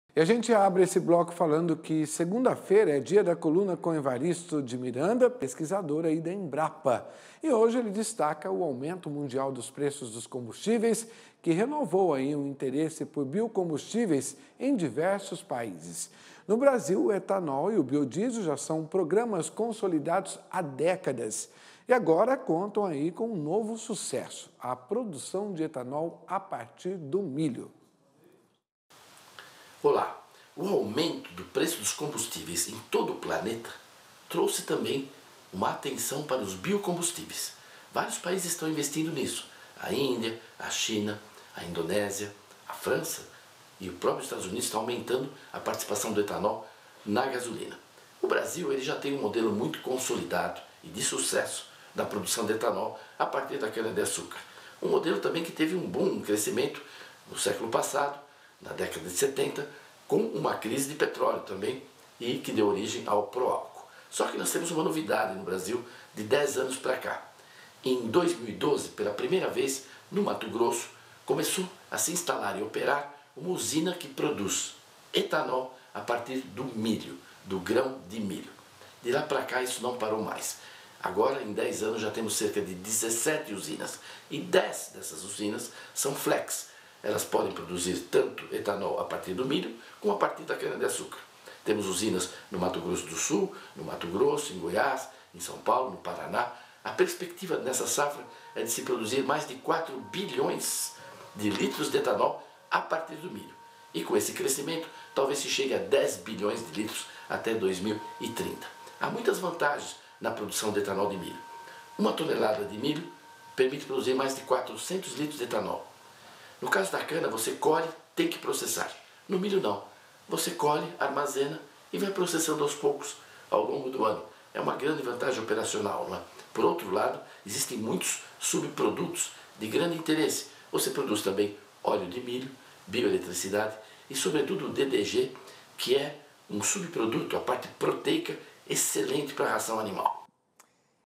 Demanda sobre o etanol de milho no Brasil
Segunda-feira é dia da coluna com Evaristo de Miranda, pesquisador da Embrapa. E hoje ele destaca o aumento mundial dos preços dos combustíveis que renovou um interesse por biocombustíveis em diversos países.No Brasil, o etanol e o biodiesel já são programas consolidados há décadas. E agora, contam com um novo sucesso: a produção de etanol a partir do milho. I AgroMais